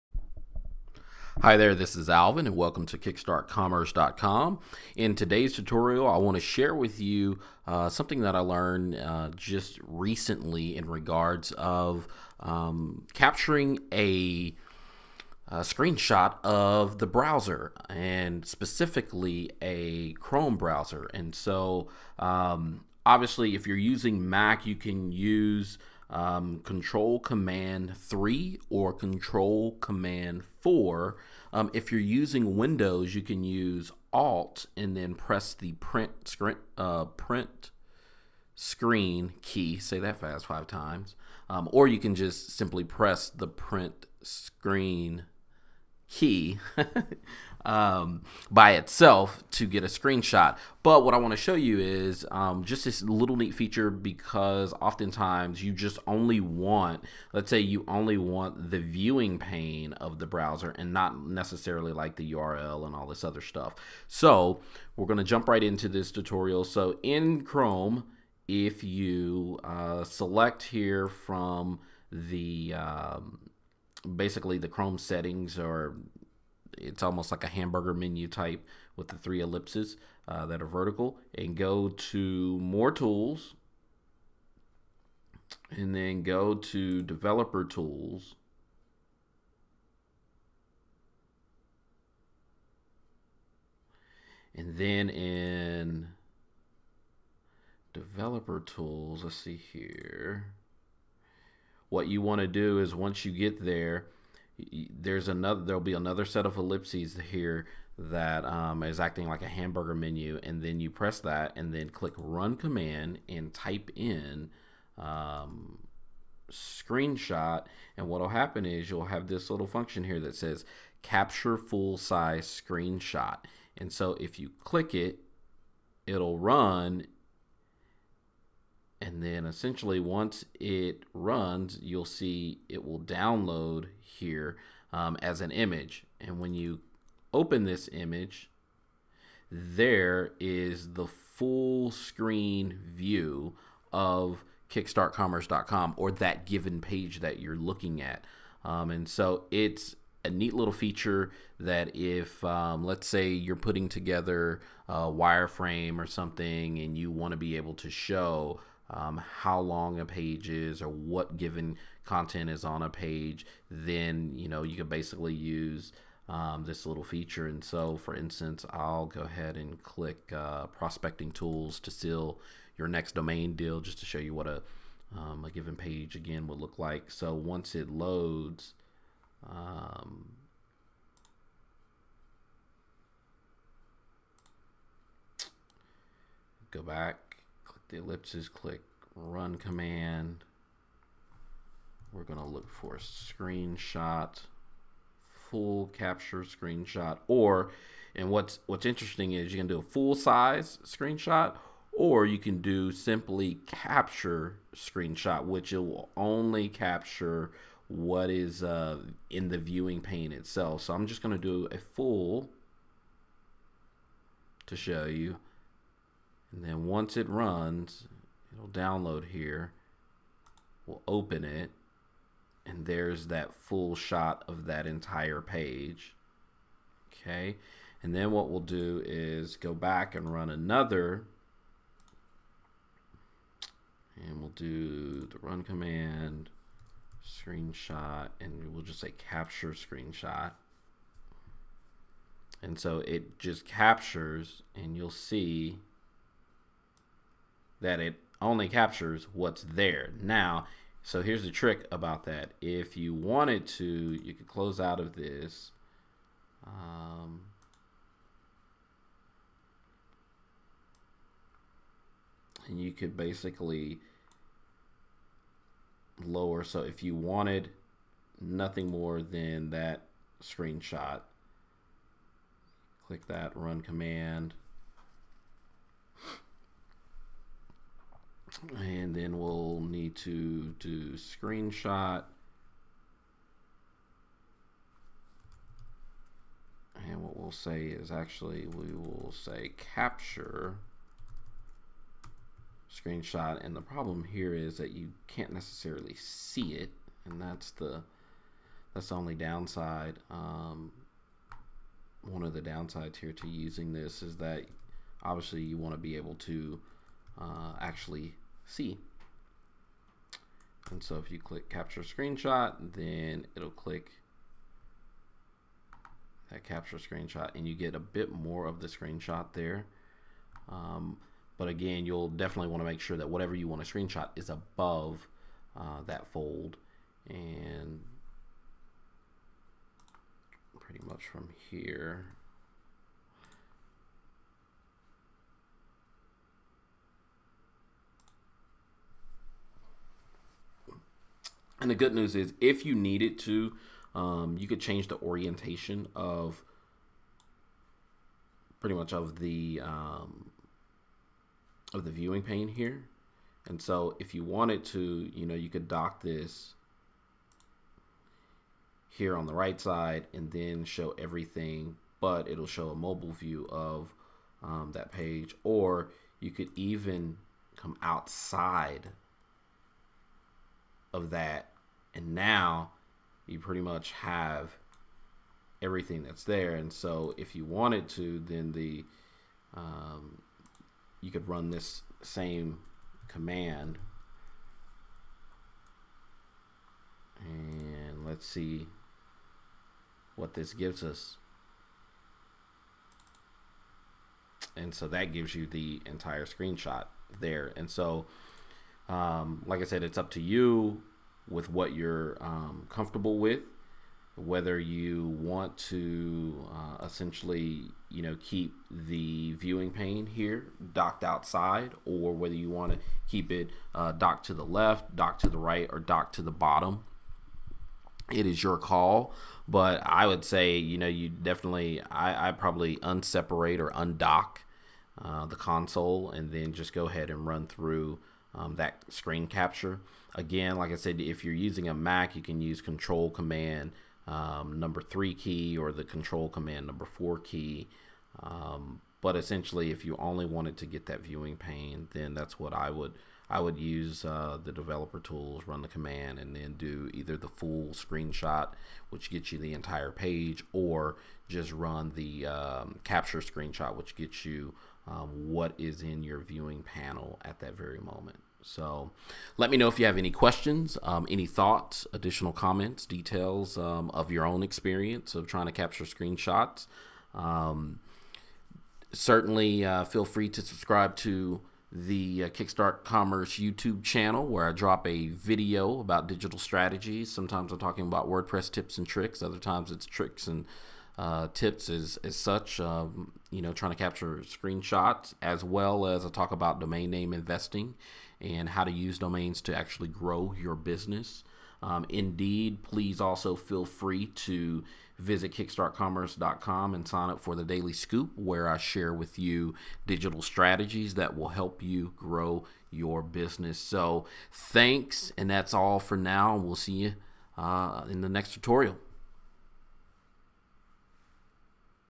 Tutorial: Screenshot using Google Chrome’s Developer Tools
While most people likely use a 3rd-party software, native Windows Print Screen or Mac screenshot keystrokes, or web browser add-ons, most web users are in the dark on the built-in screenshot capabilities offer in Google Chrome’s Developer Tools.

If you’re not familiar with Google Chrome’s Developer Tools, then you’re missing out on a trove of built-in web browser capability when it comes to capturing screenshots, area screenshots, node screenshots, and full screenshots.

Watch this quick tutorial as I demonstrate how easy and fast it is to create all sorts of screenshots using Google Chrome’s Developer Tools.

If you have questions or comments about creating screenshots Google Chrome’s Developer Tools, or general screenshot inquiries, please drop me a comment or question below, and I’ll do my best to find the appropriate answer.